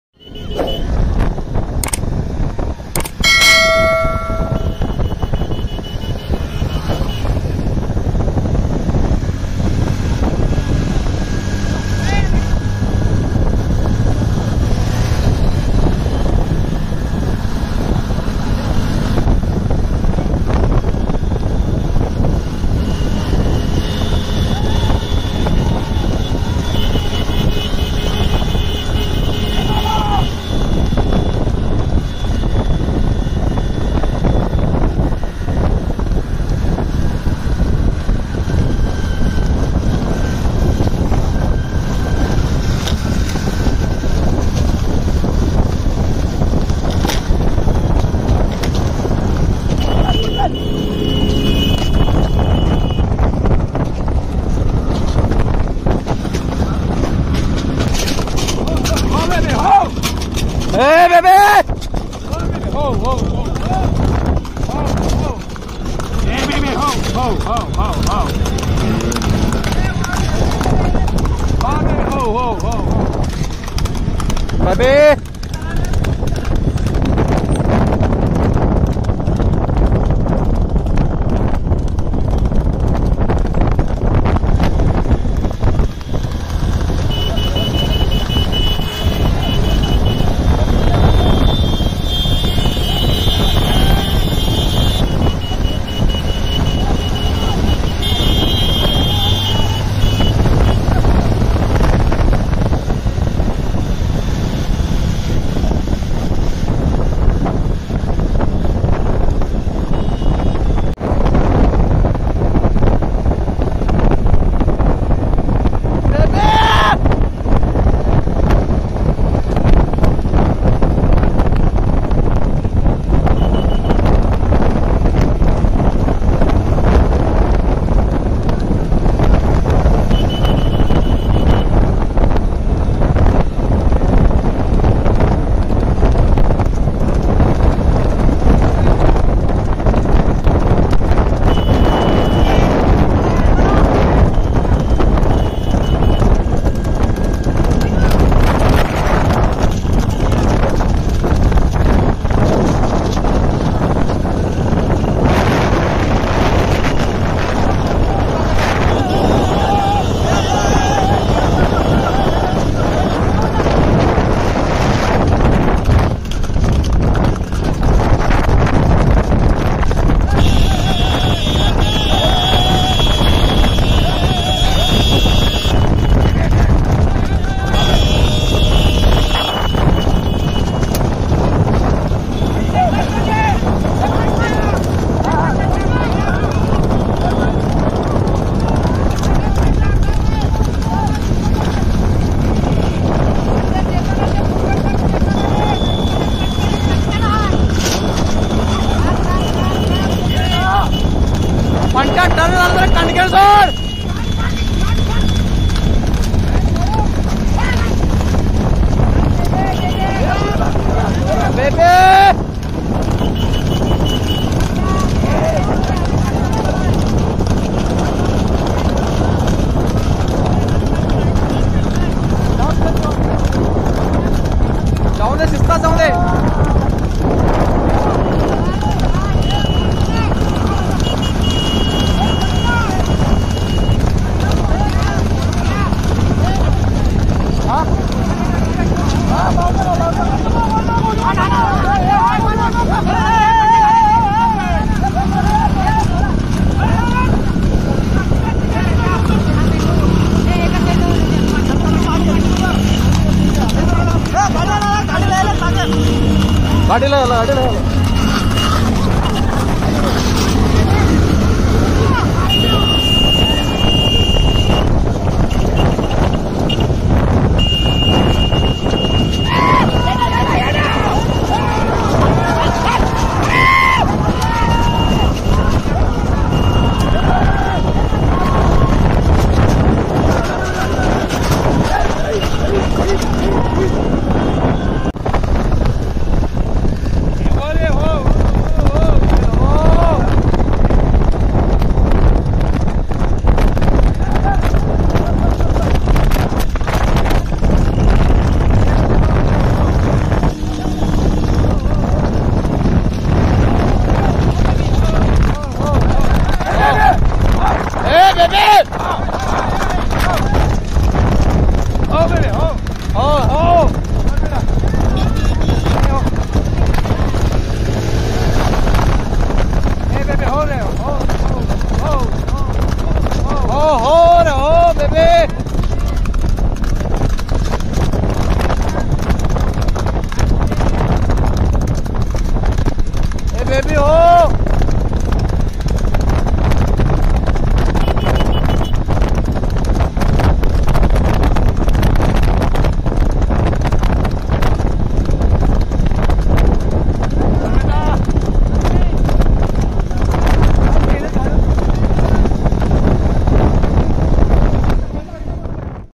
🔴शेंडयाचा जनावर दहशत किंग वाळवा बेबी👑🔥 कसला वेग आहे ✌️ घोडाग़ाडी शर्यत सौंदती horse cart race
*🏆 सौंदती नवतर जनरल निकाल🏆*

*⛔ बक्षिस :- 11,000₹*

*🥇दहशत किंग वाळवा बेबी / रणजित कुरणे शिरढोण सिकंदर*

*🔥👑 हिंदकेसरी फंट्या सुट्टी एकच 👑🔥*

*🥈कुरुंदवाड आबलक / कुपवाड पंचकल्यान*

*🔥हिंदकेसरी गेणु आण्णा🔥*

*🥉 अक्षय रजपुत डंगर / जावेद कुरणे मारी*

*🔥हिंदकेसरी सूमित सांगलवाडी🔥*

🔴शेंडयाचा_जनावर दहशत_किंग वाळवा_बेबी👑🔥 कसला वेग आहे ✌️ घोडाग़ाडी_शर्यत सौंदती